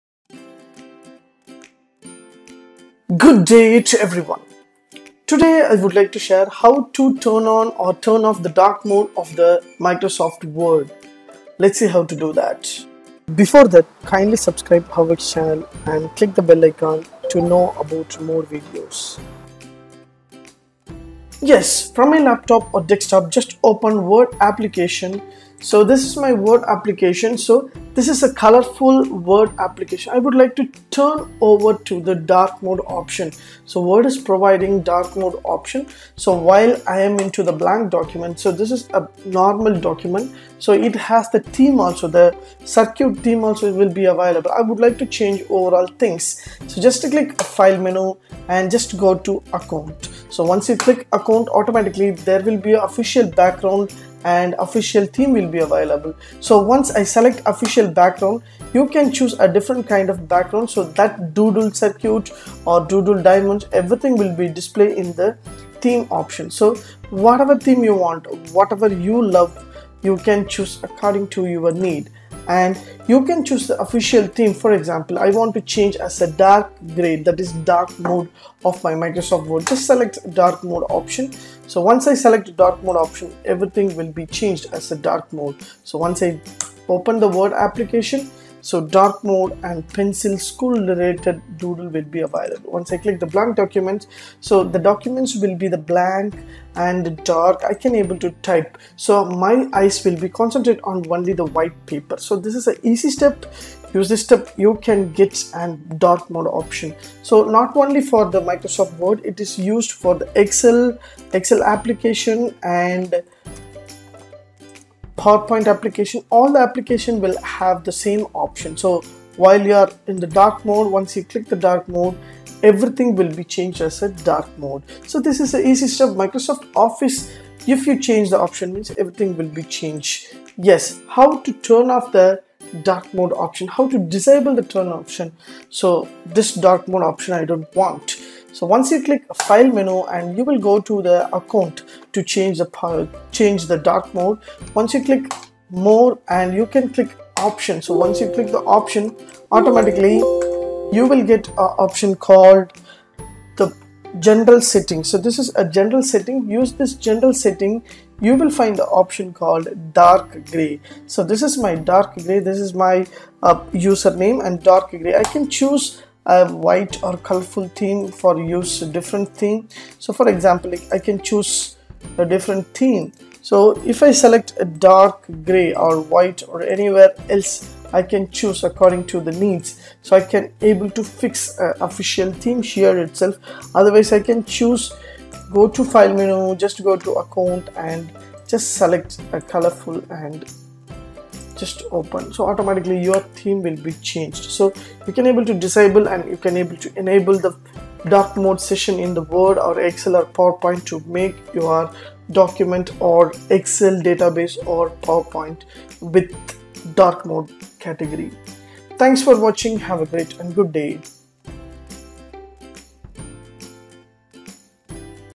How to turn on/off the dark mode in MS word, Excel and PPT 2021
This video will help you How to turn on/off the dark mode in MS word, Excel and PPT 2021

How to create logo for YouTube channel from mobile 2021